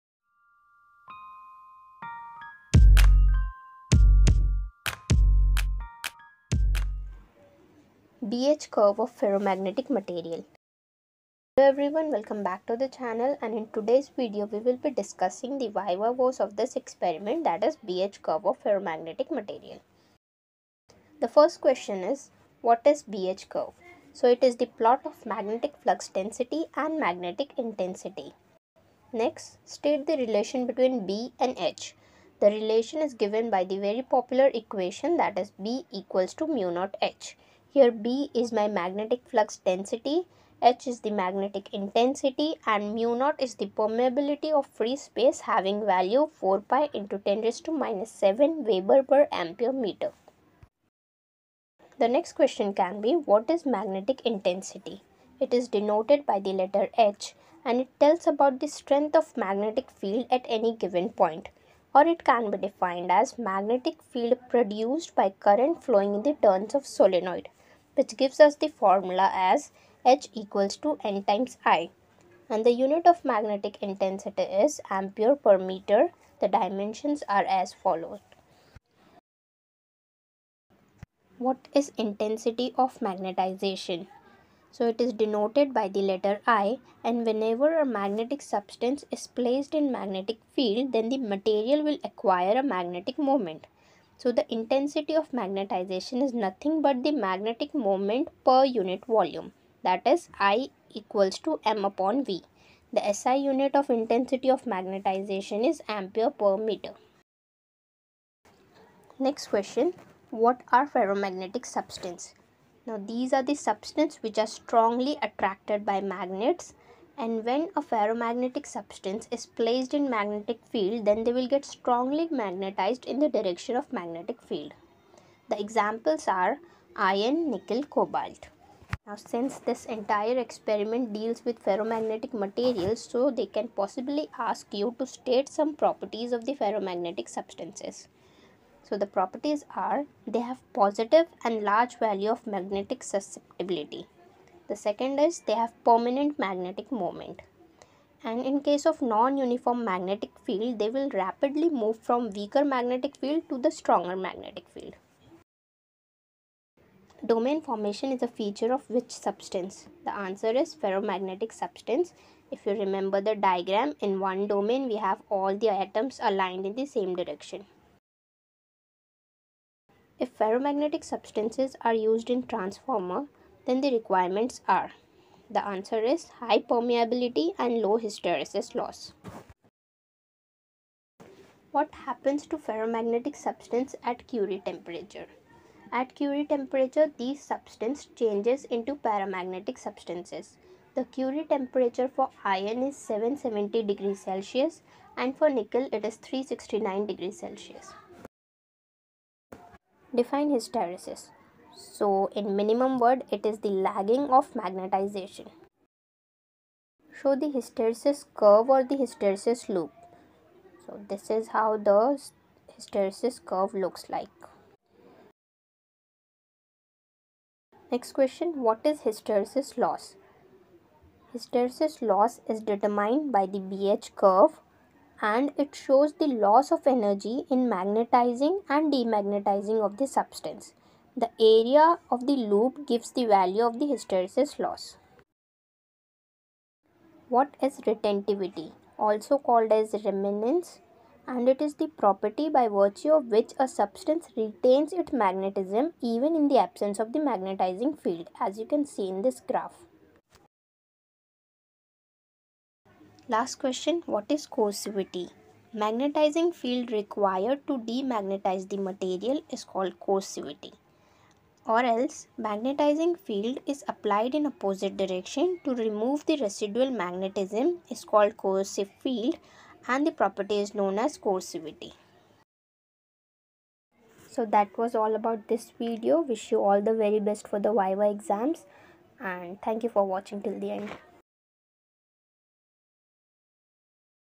V.imp! B-H curve of ferromagnetic material viva voce
Here's everything you need to know about the experiment - B-H curve of ferromagnetic material

Hey guys,

This viva voce video will help you in getting a better understanding of the experiment and gain good marks in practical viva.

We will see -
What is B-H curve?
Relation between B and H
Magnetic intensity
Intensity of magnetization
Ferromagnetic substances
Curie temperature
Hysteresis
Hysteresis loss
Retentivity
Coercivity

B-Hcurveexperimentvivavoce
B-Hcurveofferromagneticmaterialvivaquestions